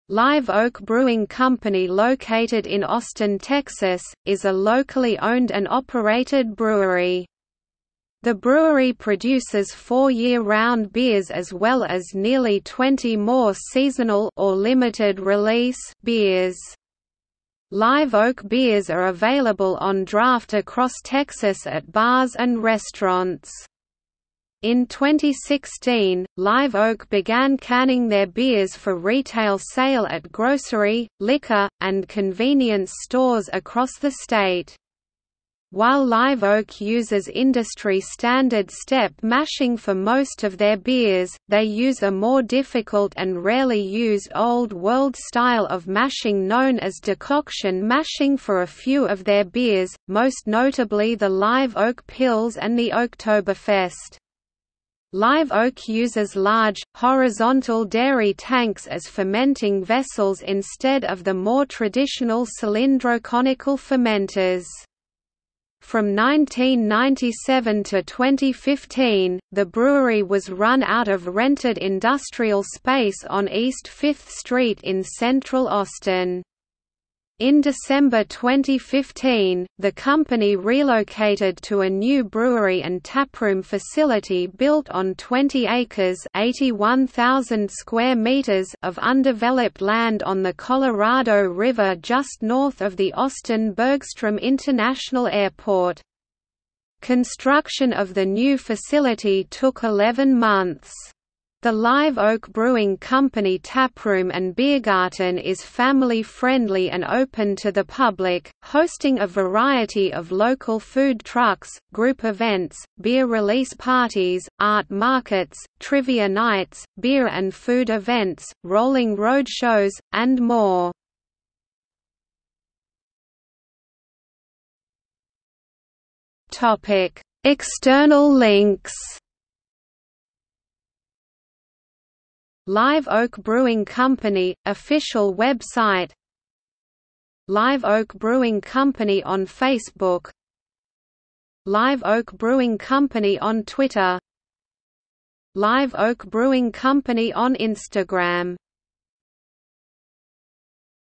Live Oak Brewing Company | Wikipedia audio article
00:02:03 External links

- Socrates

SUMMARY
Live Oak Brewing Company located in Austin, Texas, is a locally owned and operated brewery.  The brewery produces four year-round beers as well as nearly 20 more seasonal (or limited release) beers.  Live Oak beers are available on draft across Texas at bars and restaurants. In 2016, Live Oak began canning their beers for retail sale at grocery, liquor, and convenience stores across the state.
While Live Oak uses industry standard step mashing for most of their beers, they use a more difficult and rarely used old world style of mashing known as decoction mashing for a few of their beers, most notably the Live Oak Pilz and the Oaktoberfest.
Live Oak uses large, horizontal dairy tanks as fermenting vessels instead of the more traditional cylindroconical fermenters.
From 1997 to 2015, the brewery was run out of rented industrial space on East 5th Street in central Austin.  In December 2015, the company relocated to a new brewery and taproom facility built on 20 acres (81,000 m2) of undeveloped land on the Colorado river just north of the Austin-Bergstrom International Airport. Construction of the new facility took 11 months.
The Live Oak Brewing Company Taproom & Biergarten is family-friendly and open to the public, hosting a variety of local food trucks, group events, beer release parties, art markets, trivia nights, beer and food events, Rolling Roadshows, and more.